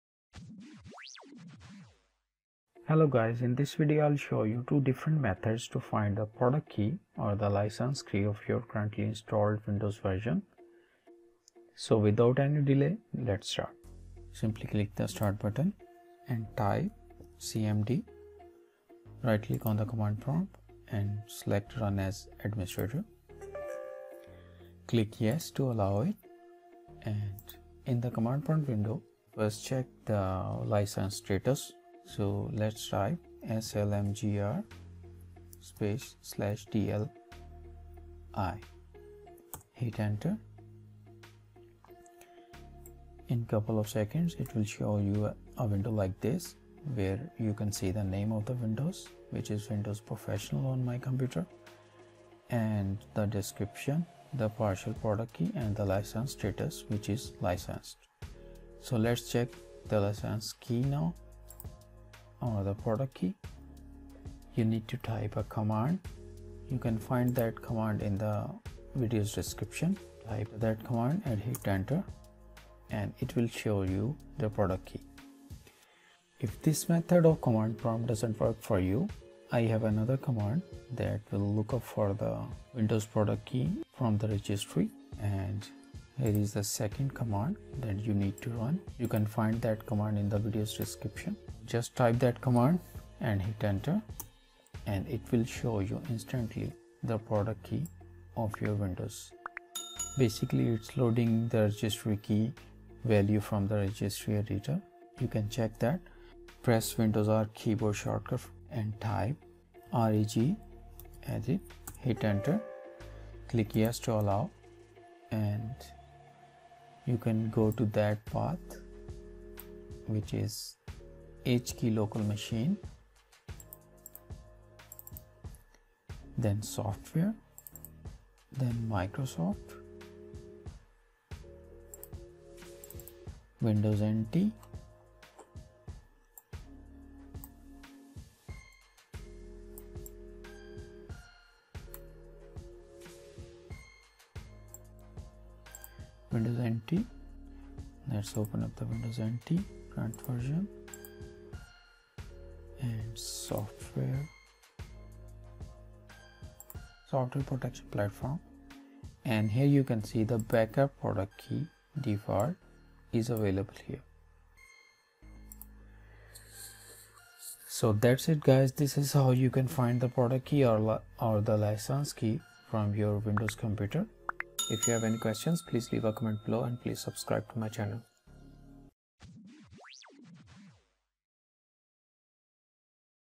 How to Find Windows Product Key from Command Prompt / Registry
In this video, you will learn how to find the Windows product key or the license key from the Windows that you are using. I will show you two commands that you can run in the command prompt to get the Windows product key.

If the wmic command doesn't work for you then you can try the second one that lookup the products key in the Windows registry.

🔸COMMAND #0🔸
To find the Windows Product ID, Name, License status, Activation ID, Application ID, Product key channel, installation id, and more
slmgr /dli
slmgr /dlv

🔸COMMAND #1🔸
On one of my computers, this command doesn't show anything. If this is the case with you then use Command #2 to get the product key from the registry.

🔸COMMAND #2🔸
Instead opening the registry editor and navigating through a list of keys and then viewing the Windows product key is a bit headache. So, I came up with this command that easily fetches and shows you the product key.

Reg Query "HKEY_LOCAL_MACHINE\SOFTWARE\Microsoft\Windows NT\CurrentVersion\SoftwareProtectionPlatform" /v BackupProductKeyDefault

🔸COMMAND #2🔸
HKEY_LOCAL_MACHINE\SOFTWARE\Microsoft\Windows NT\CurrentVersion\SoftwareProtectionPlatform" /v BackupProductKeyDefault